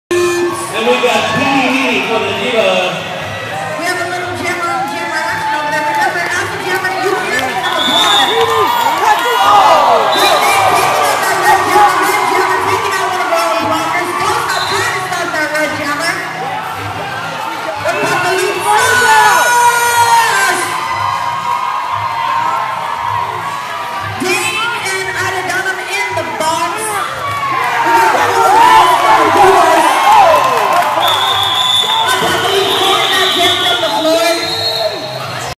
PCDD Vs IEDD 6/11/2011 - Great Dame big hit
A jam from Inland Empire Derby Dames Vs Prison City Derby Dames June 11th - In this jam, PCDD Great Dame delivers a big hit the IEDD jammer, Apocaloops gets the lead jammer status. On scoring pass Americannon hits Apocaloops down so she calls off the jam scoring 3 points.